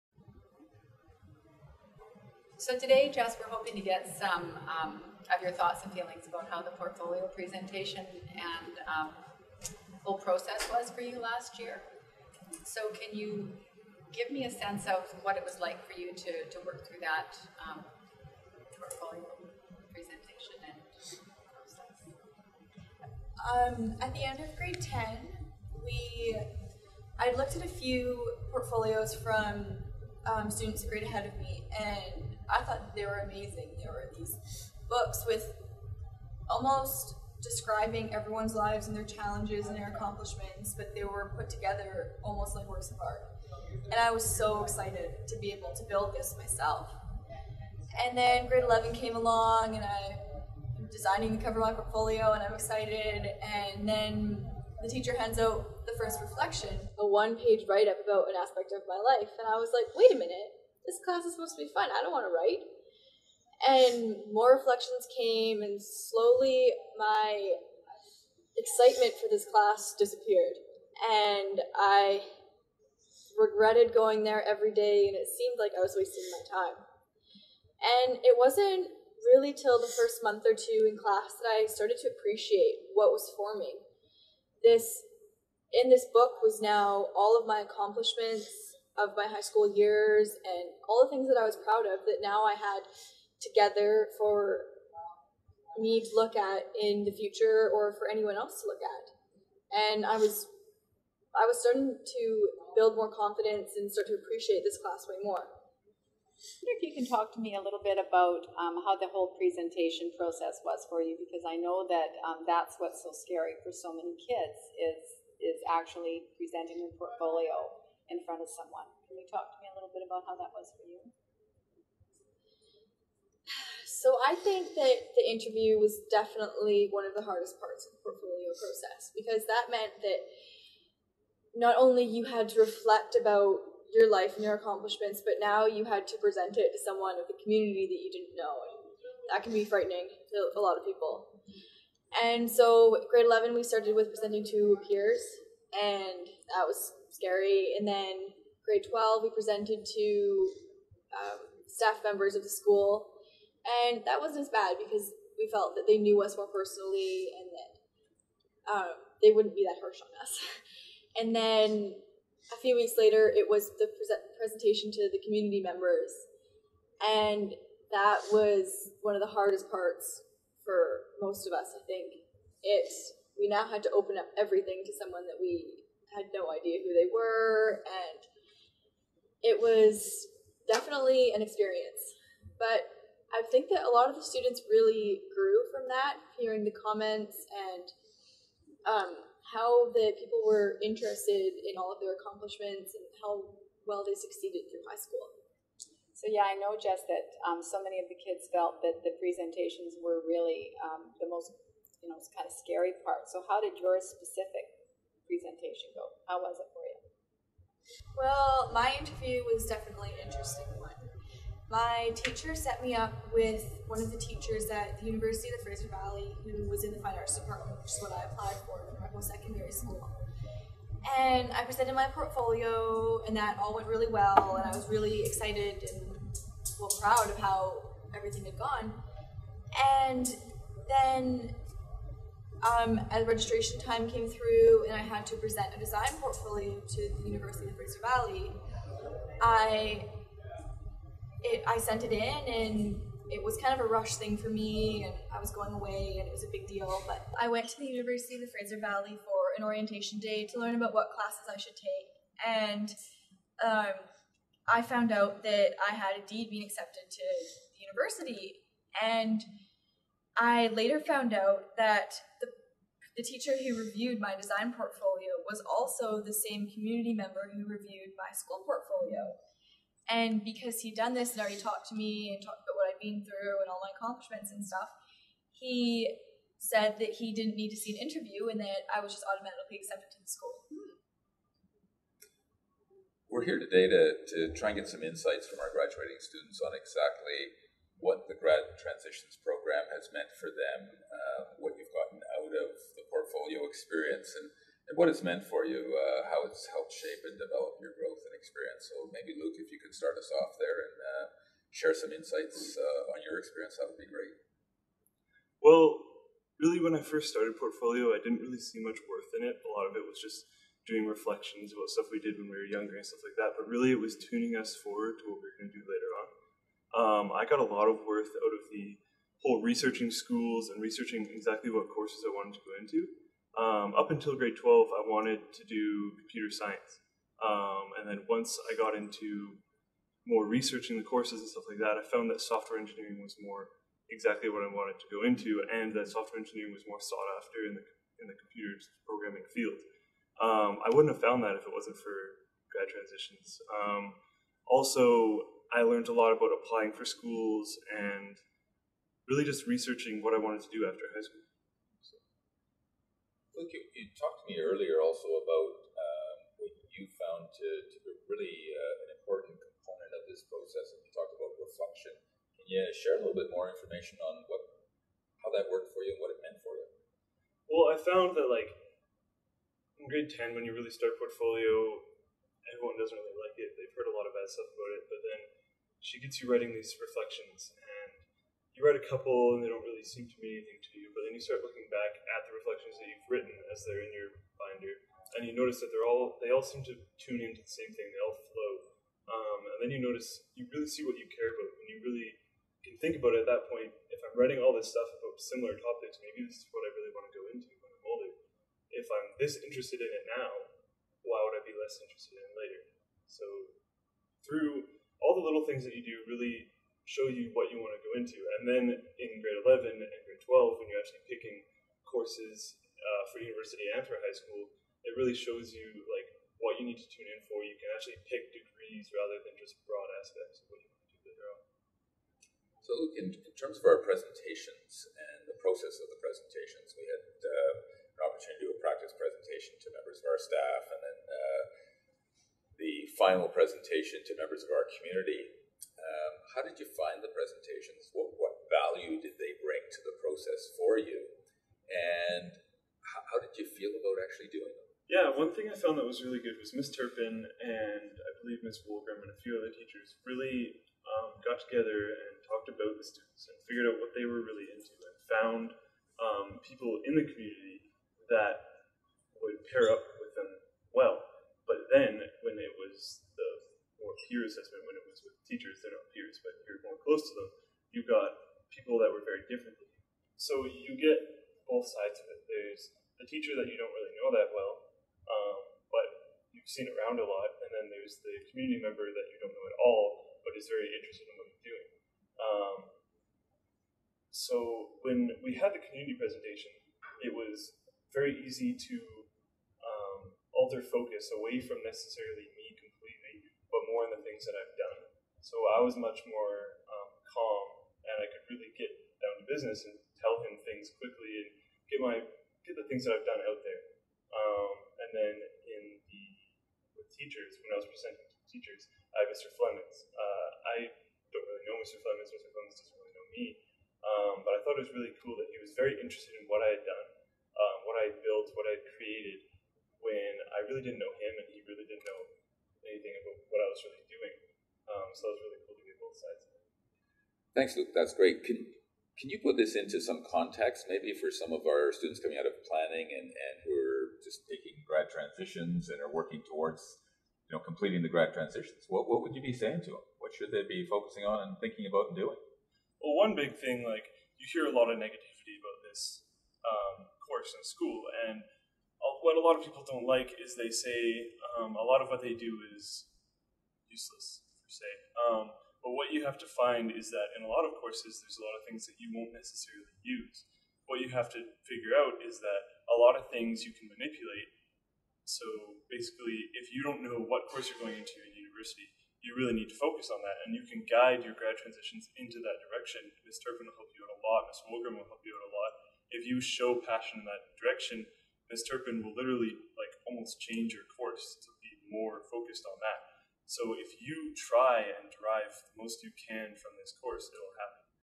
Portfolio Interviews - D.W. Poppy Secondary School
A pair of Poppy students divulge their "Portfolio" experience, its immense benefits, and the beneficial journey it has proven to be.